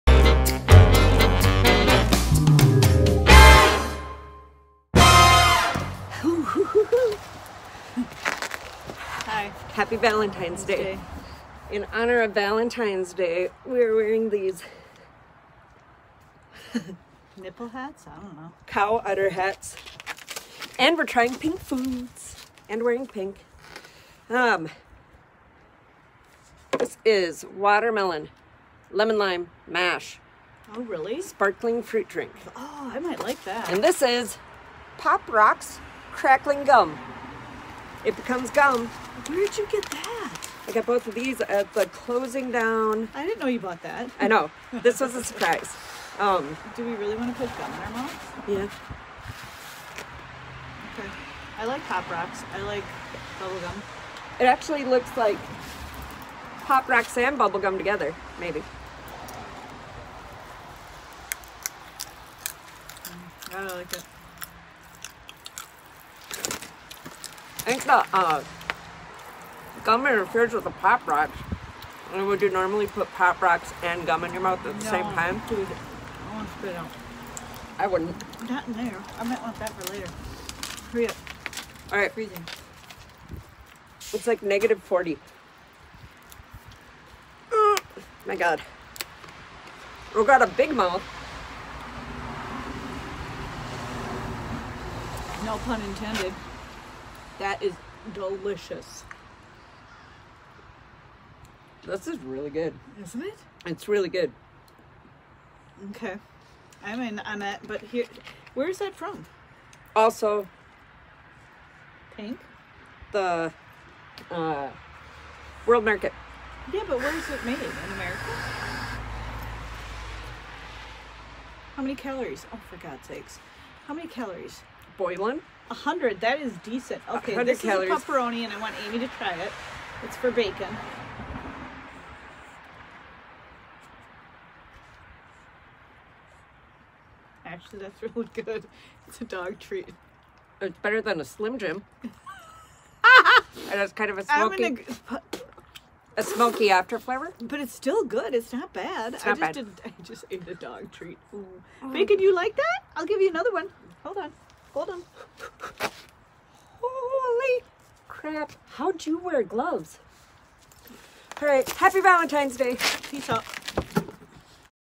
Pop Rocks Gum, Mash Drink, Pupperoni - You Me Food Review
Happy Valentine’s Day! Celebrating with pink & red: Pop Rocks Gum, Mash Sparkling Drink, Pupperoni!

Opening to video is made with iMovie. If you like it / use it!